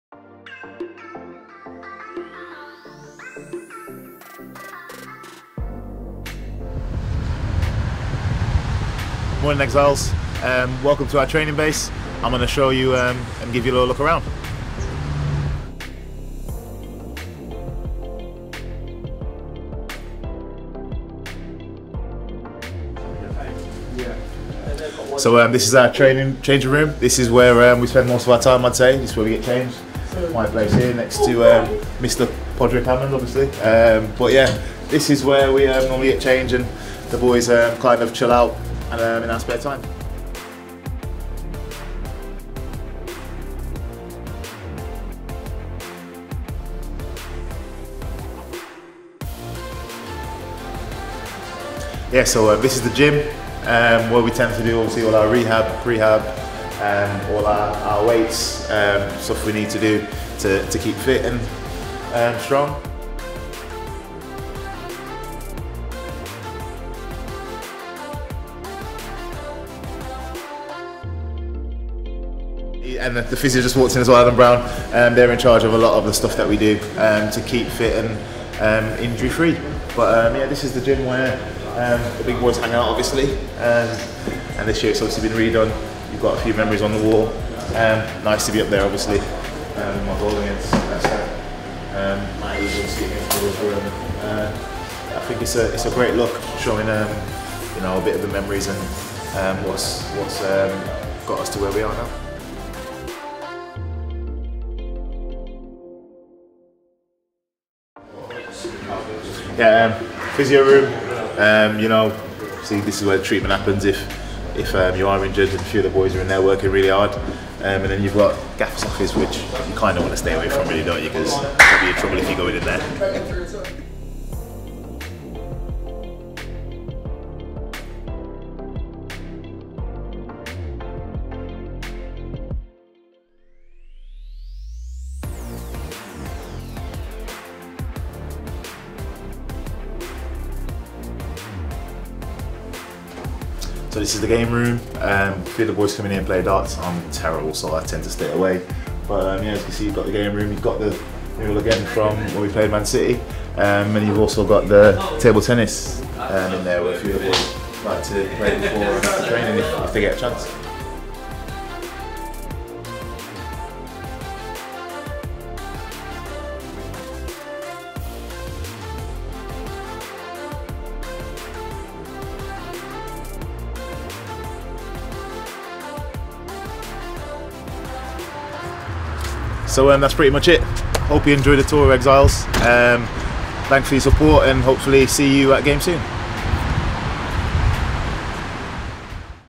TRAINING BASE TOUR | Striker Jamille Matt Shows us around the Recently ReDeveloped Training Base
Watch Striker Jamille Matt take us around the old Supporters' 'Bar Amber' grounds which is now NCAFC's Training Base.